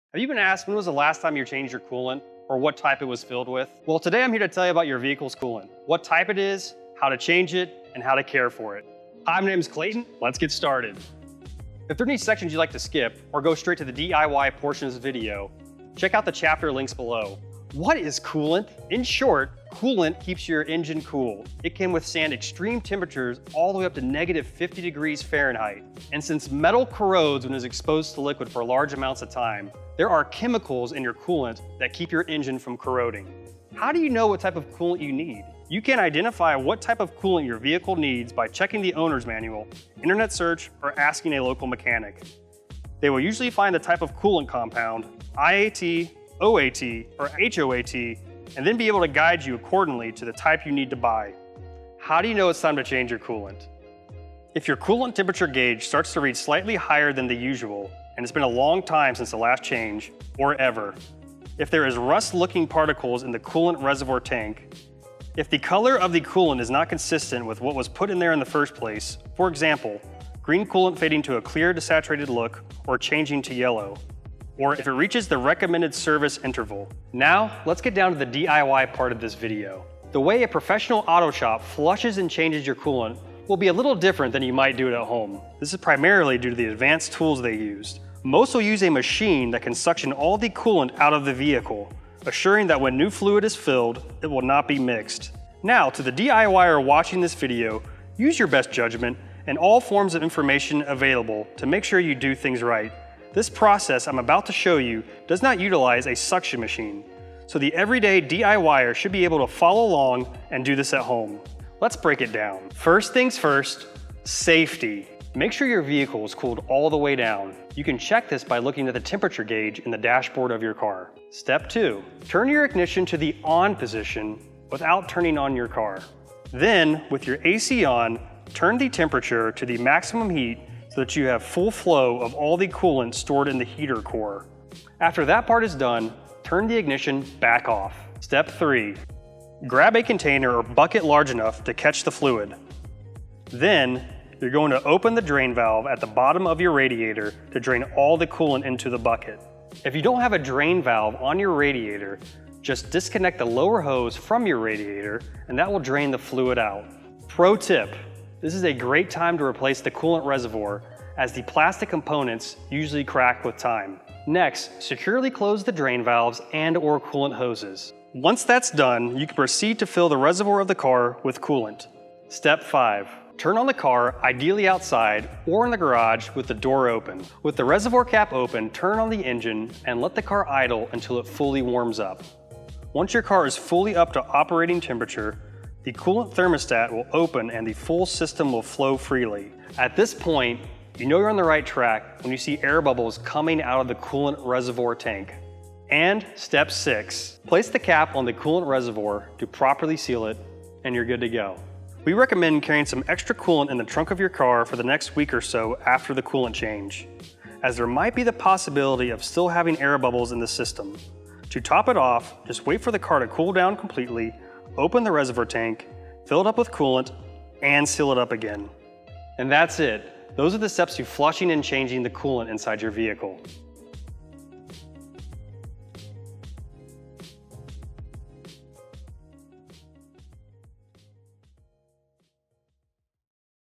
How To Flush Your Car's Coolant Correctly
FIXD helps millions of drivers diagnose the check engine light and save money on car care. In this video, you'll learn:
✅ What type of coolant your vehicle has
✅ How to change your vehicle's coolant
✅ How to care for your vehicle's coolant

Don't have FIXD yet?

0:00 Learn about car coolant
0:17 What is coolant?
0:33 What type of coolant does your car need?
0:52 When do you change your coolant?
1:17 DIY flush and change coolant
1:52 Step 1 -  Safety
2:01 Step 2 - Turn your ignition to the on position
2:17 Step 3 - Drain your coolant
2:46 Step 4 - Securely close your drain valve
2:52 Step 5 - Turn on your car
3:16 Step 6 - Close your coolant tank
3:21 After flush recommendation
3:45 Happy and safe driving!